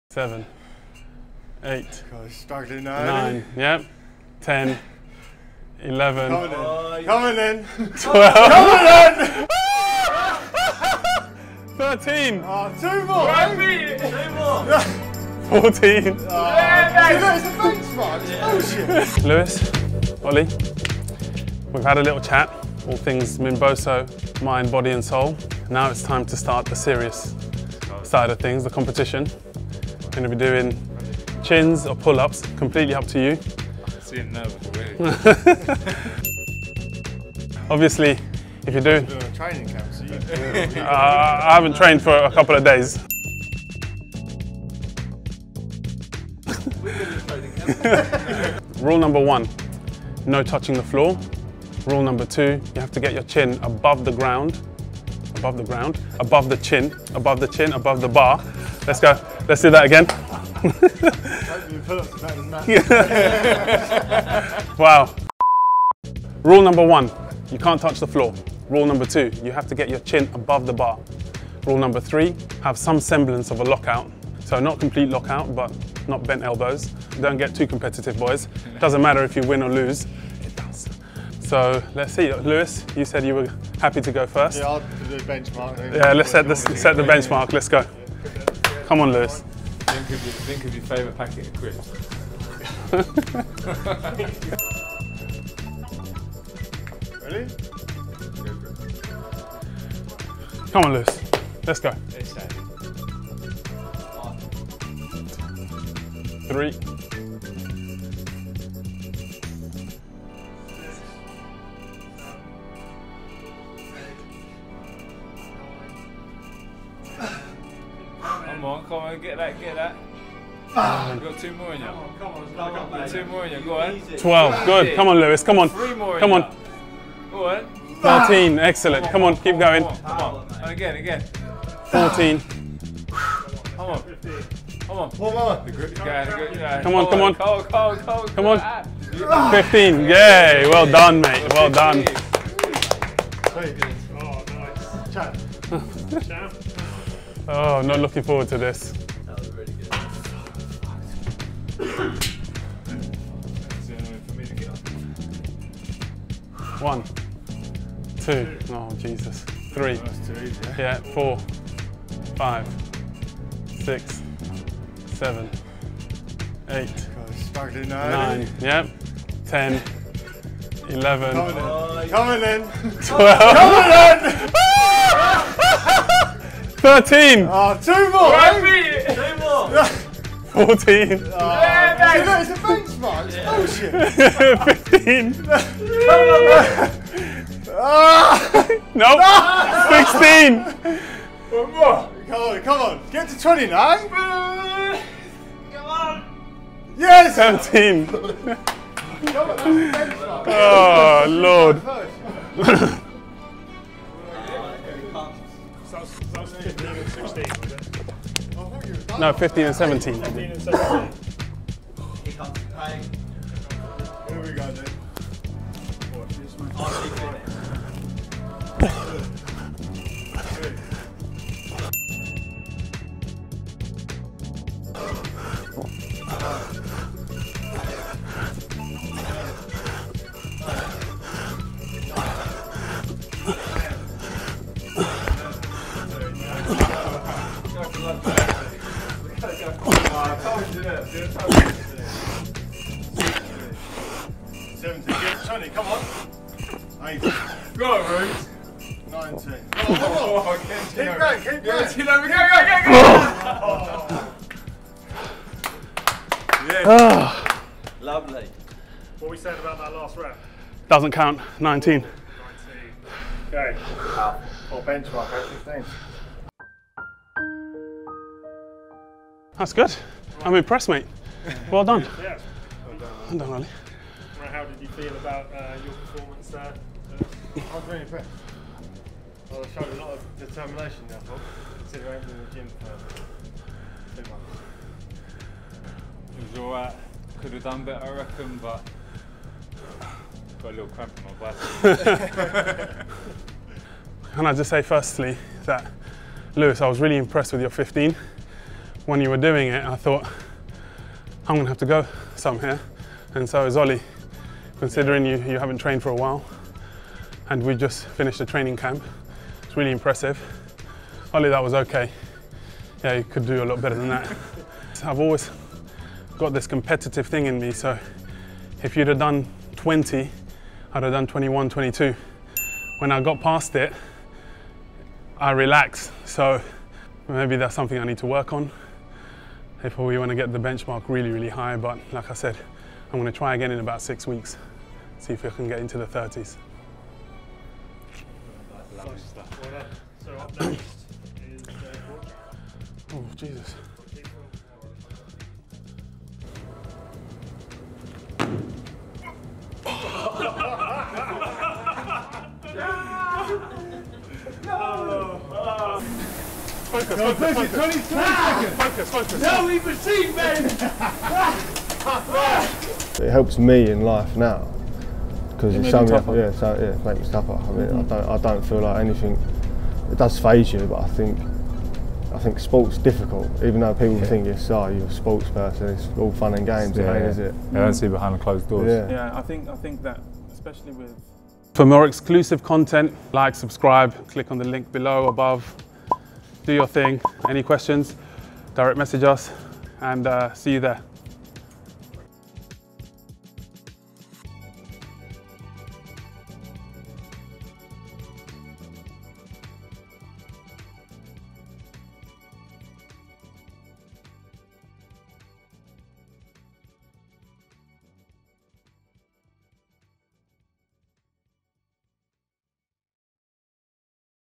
Minboso Chin Up Challenge With Oli Edwards, Ruben Tabares & Lewis Burton
Let the challenge begin!!

The Chin Up Challenge kicks off with Oli, Lewis and Ruben. All three go head to head to see who can smash out the most "Chin Above Bar" reps. The leaderboard will update every week as new guests join the challenge!

Rule 1 - Don't Touch The Ground: Once the challenge has started, as soon as you touch the ground that's it.

Rule 2 - Chin Above The Bar: Each rep has to get your chin above the bar. No chin above bar = No Rep.

Rule 3 - Get Good Extension: The rep needs to have a good extension with a partial lockout.